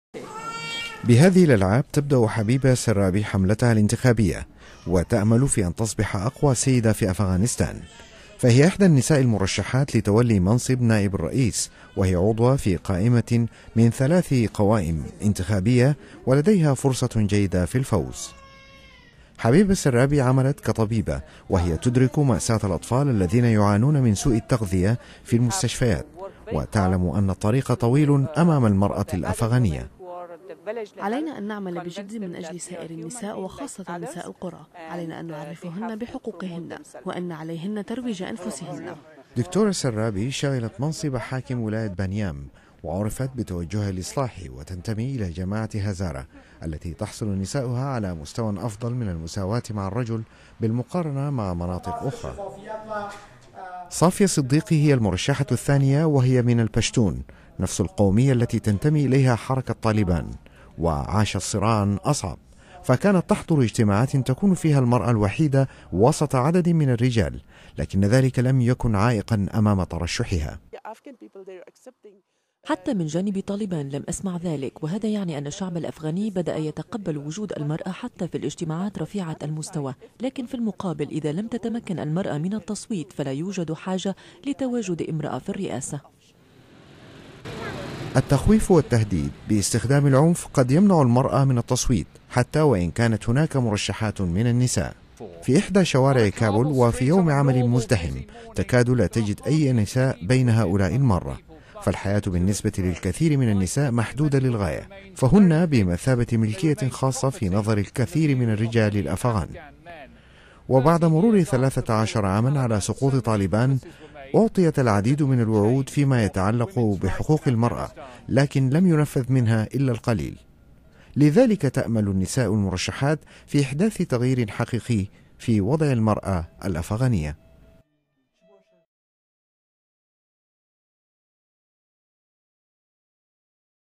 ترشيح امرأة نائبة للرئيس لأول مرة في أفغانستان
للمرة الأولى في أفغانستان، توجد فرصة أن تصبح امرأة نائبا للرئيس. فهناك ثلاث نساء مرشحات لمنصب نائب الرئيس: واحدة منهن في فريق مرشح للفوز في انتخابات السبت المقبل.